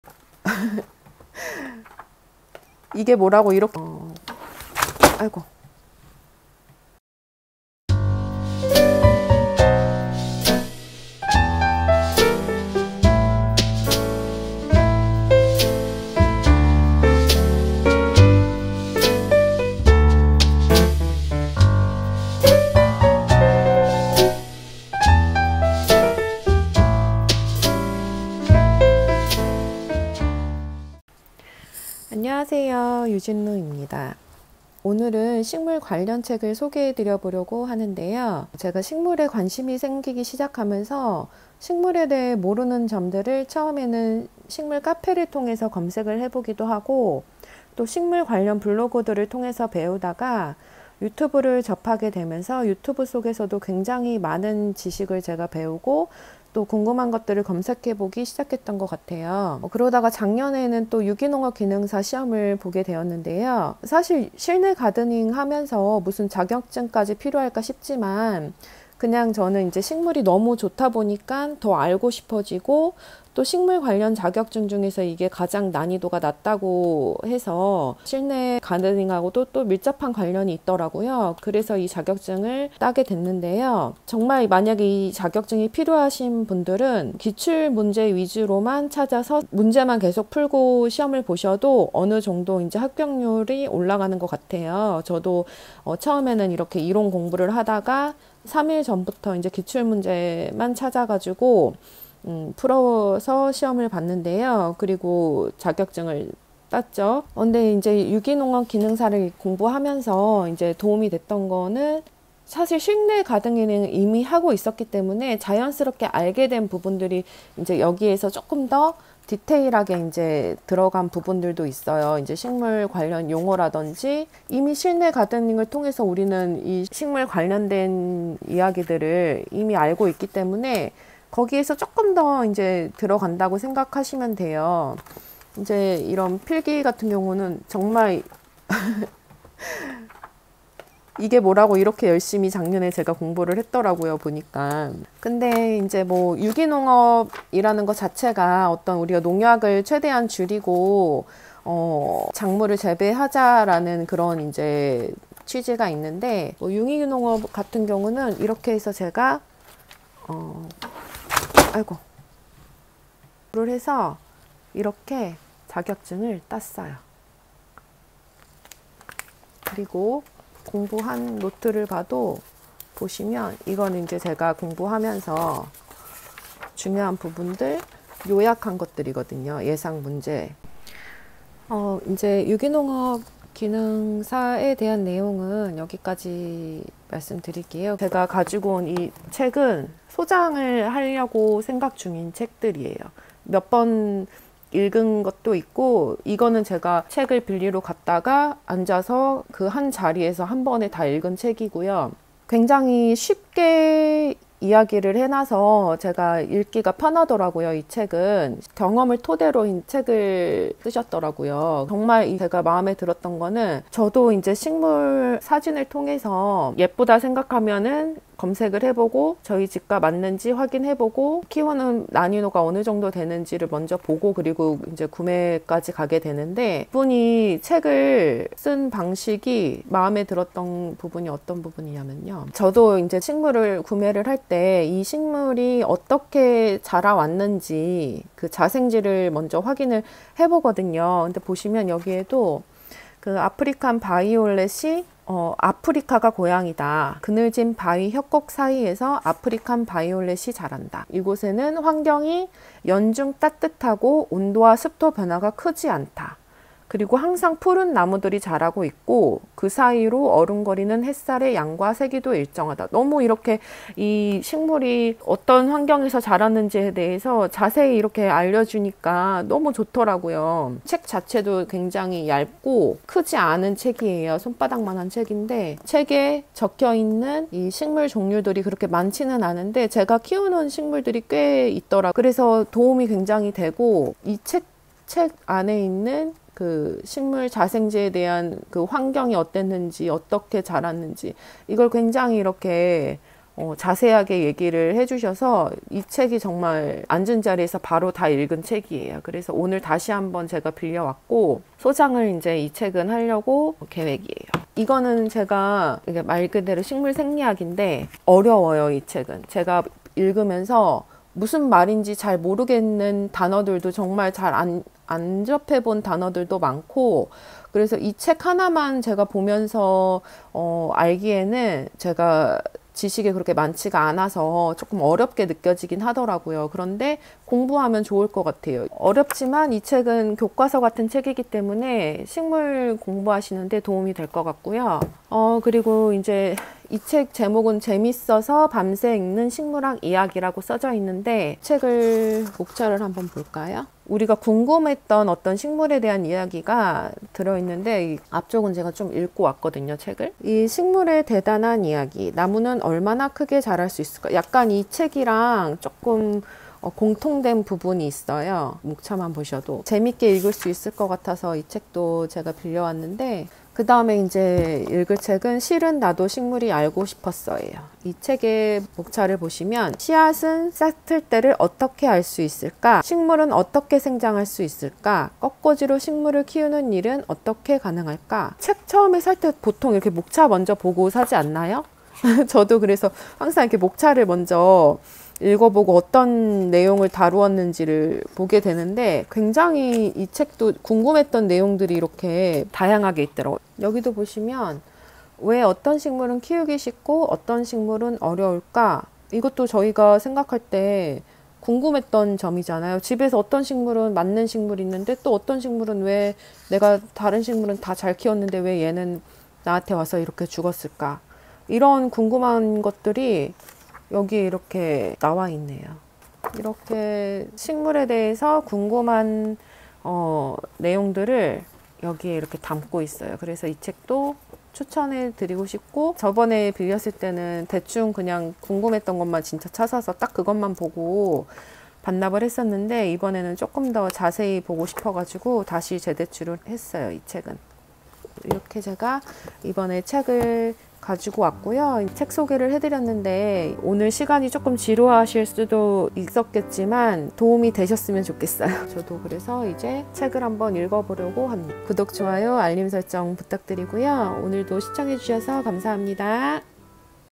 식물 키우면서 도움 되는 책 소개📗(feat.유기농업기능사)
식물의 위로 - 박원순
식물의 죽살이 - 이유
재밌어서 밤새 읽는 식물학 이야기 - 이나가키 히데히로
실은 나도 식물이 알고 싶었어 - 안드레아스 바를라게
유기농업기능사 자격증